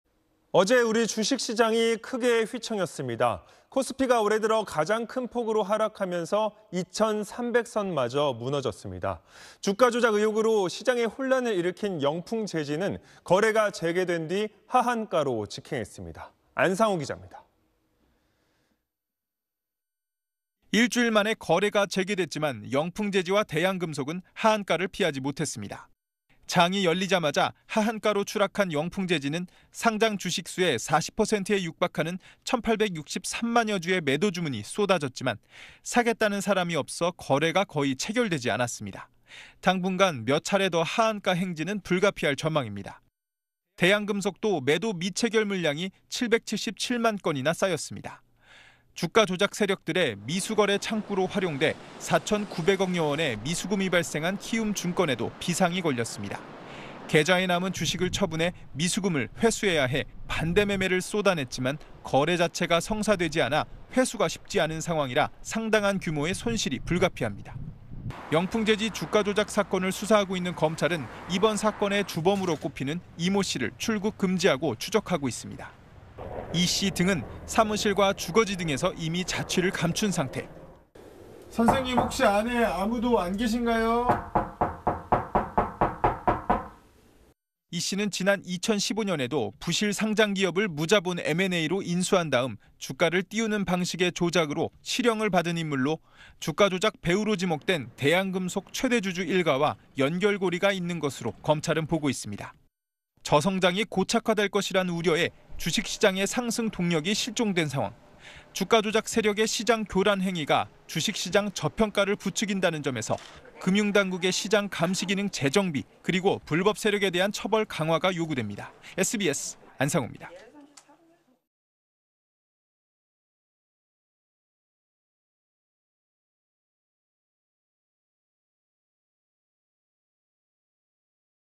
영풍제지 '하한가' 직행…주가조작에 시장 교란까지 / SBS
〈앵커〉

어제(26일) 우리 주식시장이 크게 휘청였습니다. 코스피가 올해 들어 가장 큰 폭으로 하락하면서, 2천300선 마저 무너졌습니다. 주가조작 의혹으로 시장에 혼란을 일으킨 영풍제지는 거래가 재개된 뒤 하한가로 직행했습니다.

안상우 기자입니다.

〈기자〉

일주일 만에 거래가 재개됐지만, 영풍제지와 대양금속은 하한가를 피하지 못했습니다.

장이 열리자마자 하한가로 추락한 영풍제지는 상장 주식 수의 40%에 육박하는 1863만여 주의 매도 주문이 쏟아졌지만, 사겠다는 사람이 없어 거래가 거의 체결되지 않았습니다.

당분간 몇 차례 더 하한가 행진은 불가피할 전망입니다.

대양금속도 매도 미체결 물량이 777만 건이나 쌓였습니다.

주가조작 세력들 미수 거래 창구로 활용돼 4천9백억여 원의 미수금이 발생한 키움증권에도 비상이 걸렸습니다.

계좌에 남은 주식을 처분해 미수금을 회수해야 해 반대매매를 쏟아냈지만, 거래 자체가 성사되지 않아 회수가 쉽지 않은 상황이라 상당한 규모의 손실이 불가피합니다.

영풍제지 주가조작 사건을 수사하고 있는 검찰은 이번 사건 주범으로 꼽히는 이 모 씨를 출국 금지하고 추적하고 있습니다.

이 씨 등은 사무실과 주거지 등에서 이미 자취를 감춘 상태,

[선생님. 혹시 안에 아무도 안 계신가요?]

이 씨는 지난 2015년에도 부실 상장기업을 무자본 M&A로 인수한 다음 주가 띄우는 방식의 조작으로 실형을 받은 인물로, 주가조작 배후로 지목된 대양금속 최대주주 일가와 연결 고리가 있는 걸로 검찰은 보고 있습니다.

저성장이 고착화될 거란 우려에 주식시장 상승 동력이 실종된 상황, 주가조작 세력의 시장 교란 행위가 주식시장 저평가를 부추긴다는 점에서 금융당국의 시장 감시기능 재정비, 그리고 불법세력에 대한 처벌 강화가 요구됩니다.

(영상취재 : 김세경, 영상편집 : 박기덕, VJ : 박현우)